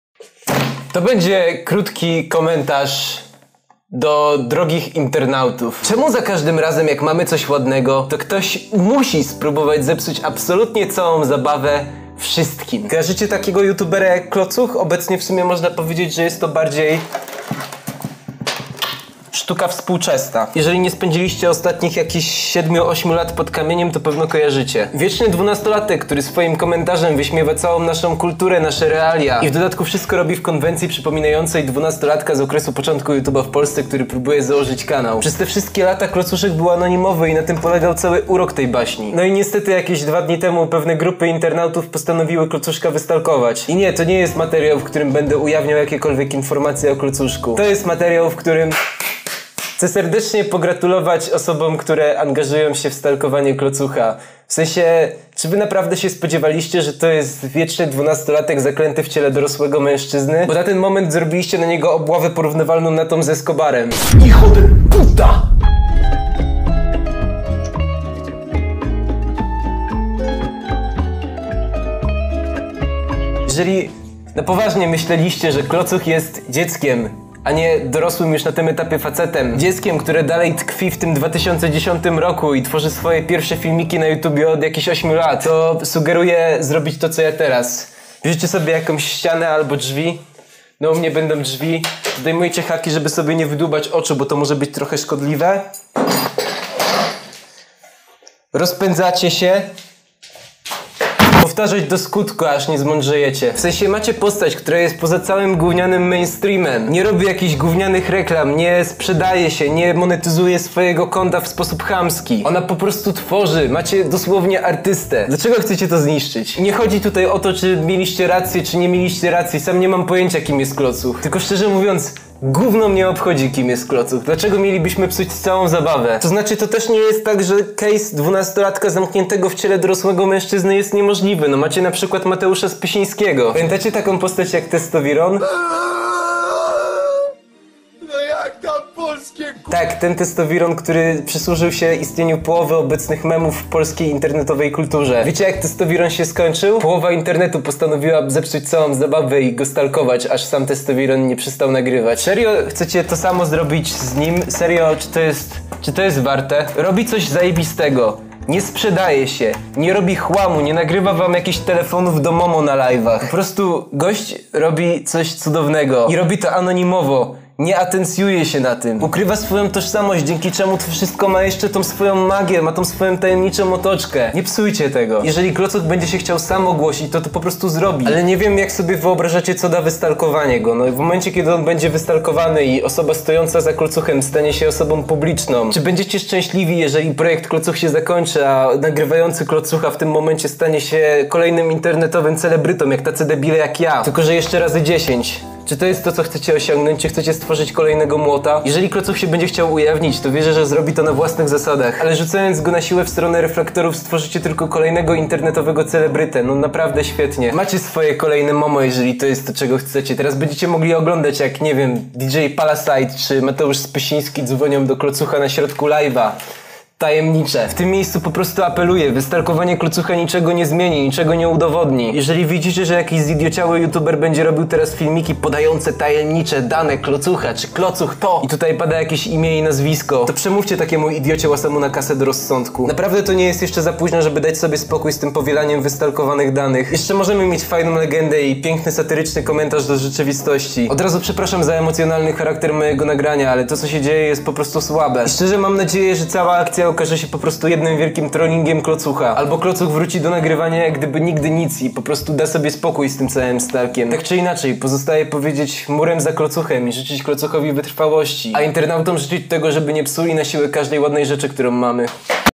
przestańcie psuć klocucha dzieki [Reupload]
Youtuber Awięc  ̶z̶b̶i̶j̶a̶ ̶h̶a̶j̶s̶ ̶n̶a̶ ̶a̶f̶e̶r̶z̶e̶ ̶z̶ ̶K̶l̶o̶c̶u̶c̶h̶e̶m̶  wstawia się za mistrzem i jego anonimową działalnością oraz oddaje mu hołd ;) A przy tym mówi parę mądrych rzeczy, dlatego reuploadujemy.
Film usunął z nieznanego powodu. (Być może dlatego, że stał się nieaktualny.) Reupload zniknie, jeśli oryginalny autor filmu sobie tego zażyczy.